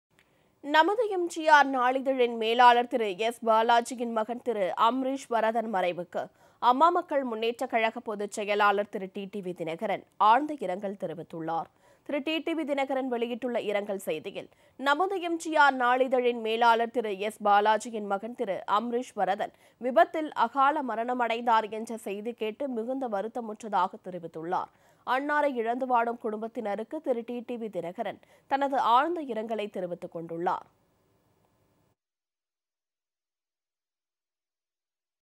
நமது எம்.ஜி.ஆர். நாளிதழில் மேலாளராக பணியாற்றும் பாலாஜியின் மகன் மறைவுக்கு டிடிவி தினகரன் இரங்கல்
டாக்டர் நமது எம்.ஜி.ஆர். நாளிதழில் மேலாளராக பணியாற்றும் பாலாஜியின் மகன் மறைவுக்கு டிடிவி தினகரன் இரங்கல்